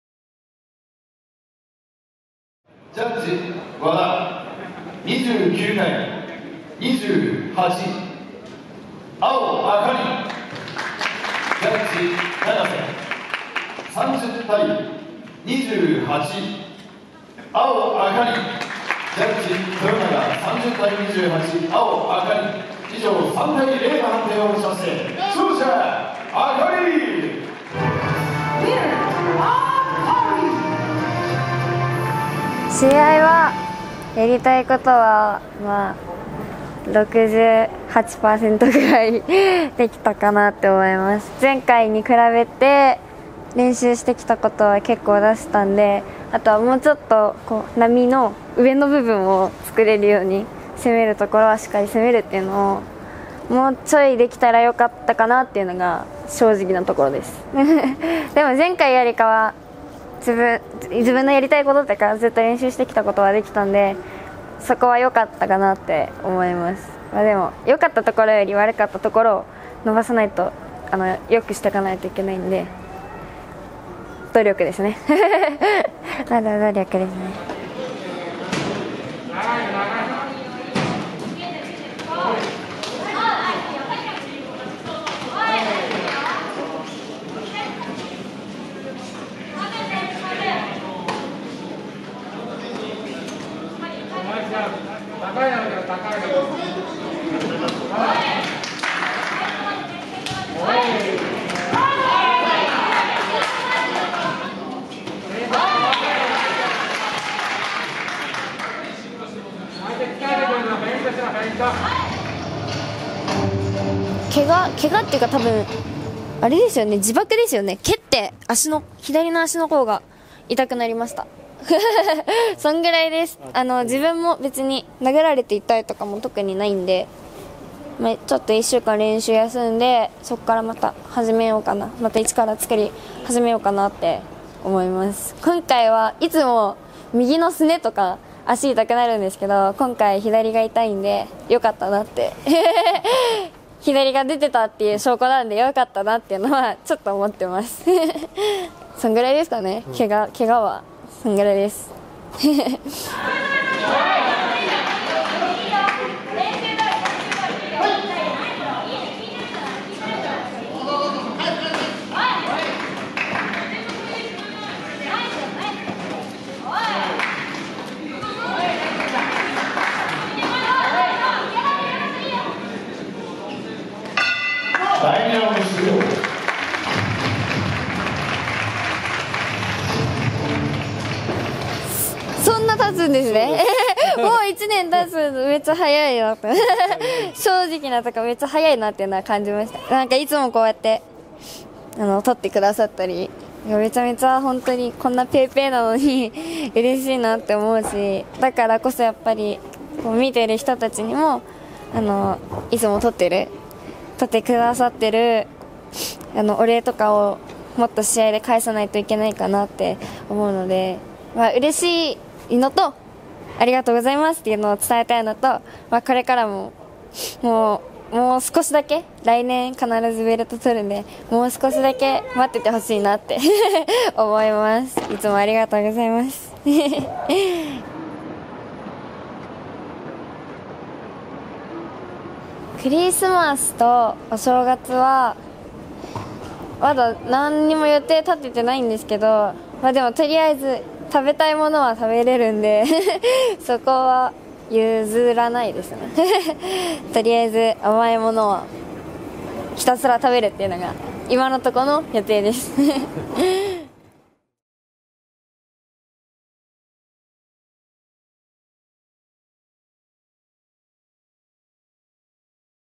Akari Episode 11 - Fifth Victory
Follow Akari who just turned 17 and won 6 fights out of 6 in her pro carrier.
Trained by the great Kamimura Erika, she's the new face of Team Target, a team that had a few champions including Nasukawa Tenshin.